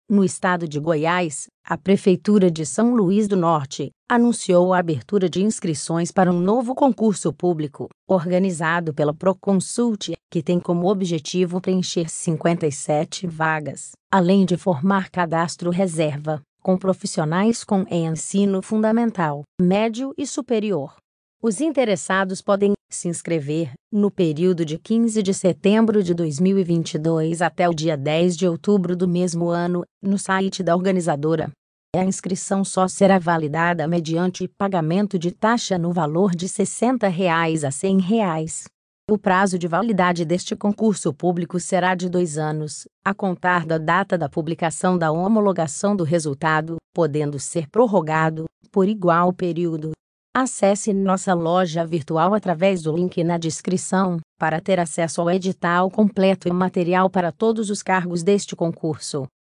Apostila PREFEITURA DE SÃO LUIZ DO NORTE GO 2022 Fiscal de Tributos
Como forma de classificação, os concorrentes serão avaliados por meio de provas objetivas previstas para 27 de novembro de 2022, redação, prova prática, prova de títulos e avaliação física de acordo com o cargo pleiteado. Informações complementares podem ser obtidas por meio do edital completo que consta em nosso site.

Os interessados podem se inscrever, no período de 15 de setembro de 2022 até o dia 10 de outubro do mesmo ano, no site da organizadora. A inscrição só será validada mediante pagamento de taxa no valor de R$60,00 a R$100,00.

➔ Cadastre-se para receber atualizações de todos os concursos 2022. Concurso CONSAMU PR 2022 👇